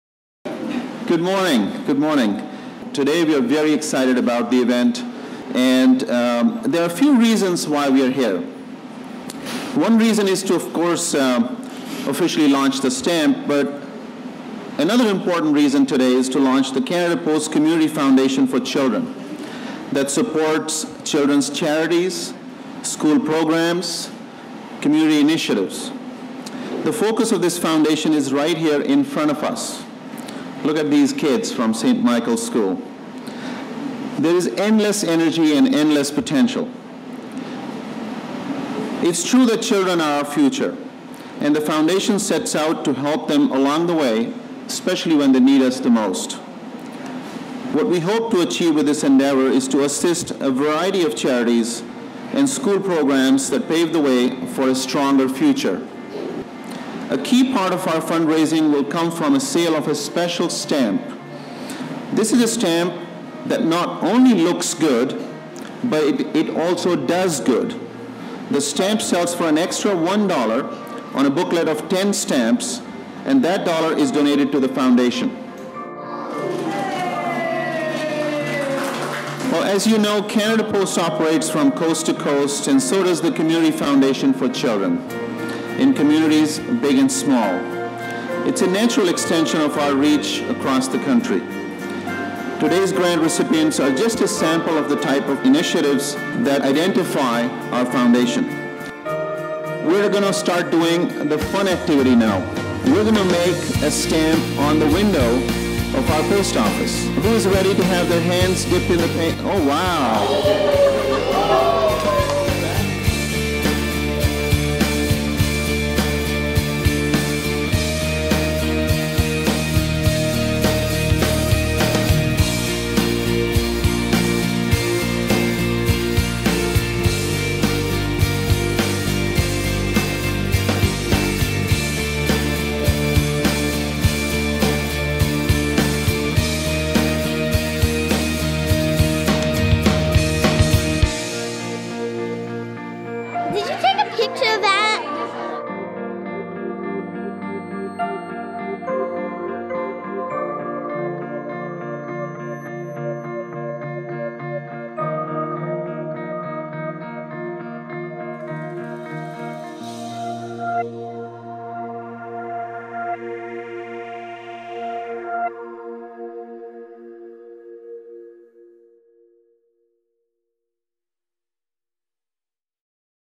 Canada Post Community Foundation
Through various programs, Canada Post employees and retirees have devoted countless hours to initiatives that improve the lives of children. By raising both awareness and funds, Canada Post will support the volunteers, and local and national non-profit groups that work every day to deliver a brighter future for Canadian children.